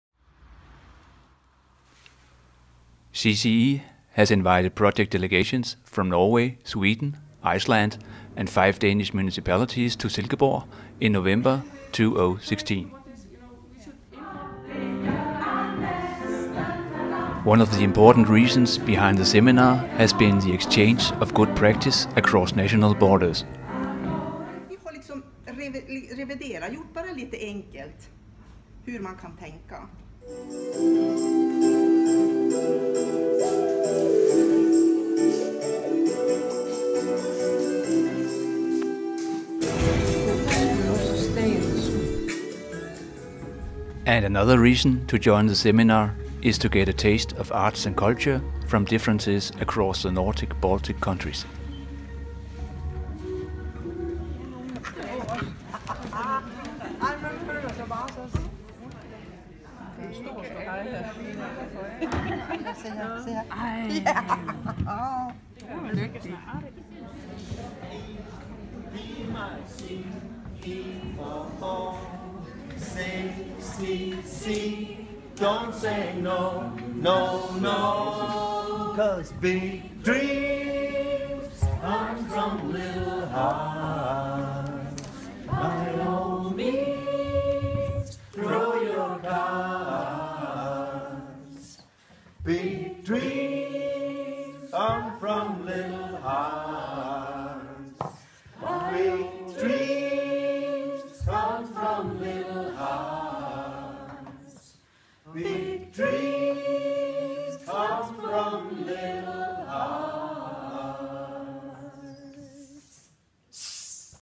CCE video
A small video presentation from our Cultural Children of Europe seminar in Silkeborg november 2016.